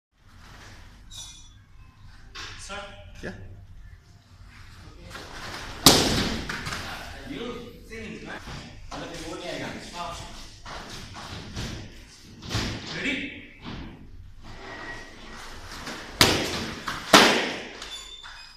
360' Kick by Sensei Rahul Ay Dubai in karate club
As a karate instructor in Golden fist international karate acedemy Dubai.